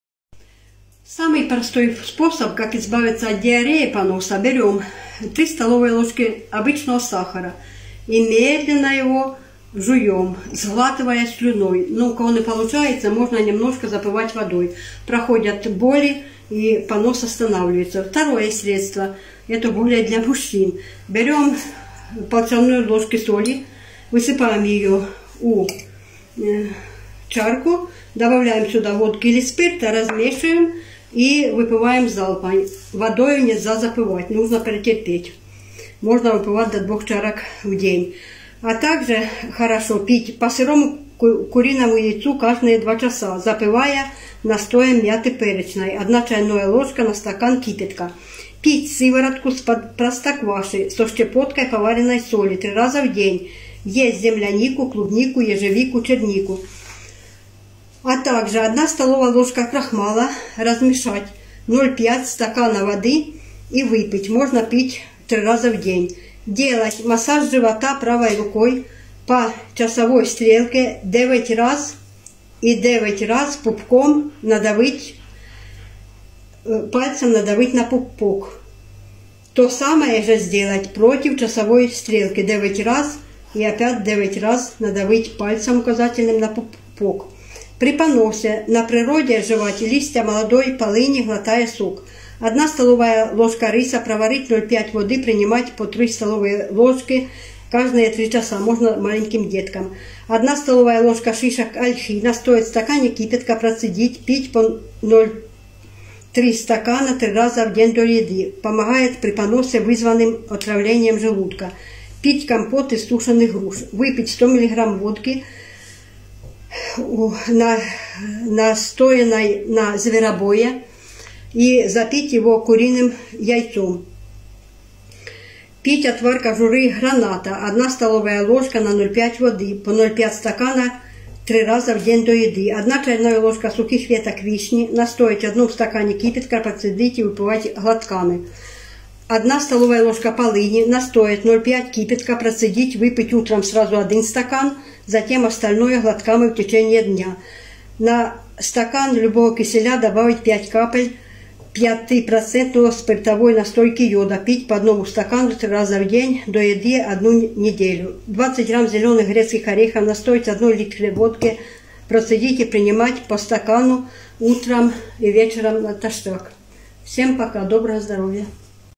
Боремся с Поносом: Необычные Методы и Народные Средства. Водка, Соль, Сахар, Крахмал и Зверобой
Я бабушка Елизавета даю вам мудрые советы.
Буду искренне благодарна каждому из вас.
Всем удачи!!!